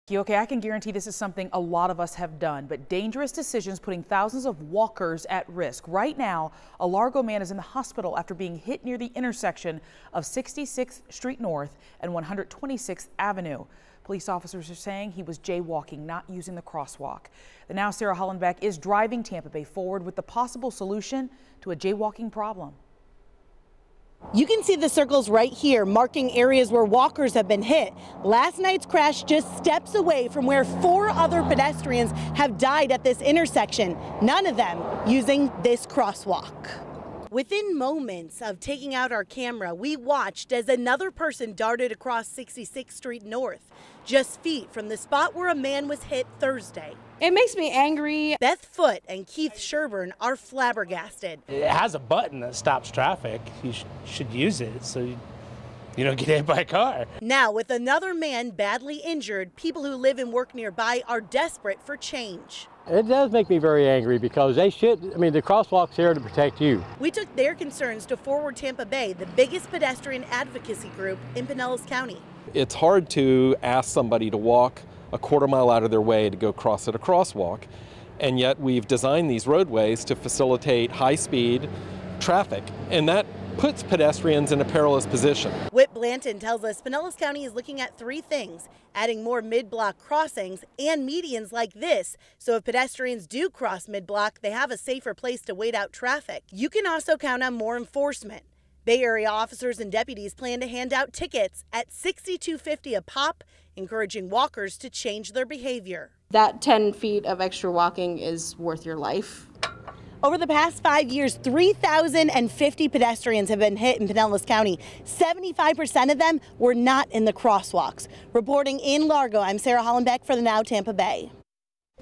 Largo residents fed up with dangerous jaywalking on 66th St N. | Driving Tampa Bay Forward
Dangerous decisions are putting thousands of walkers at risk. A Largo man remains hospitalized Friday and is fighting for his life at Bayfront Health in St. Petersburg after being hit near the intersection of 66th Street North and 126th Avenue N in Largo.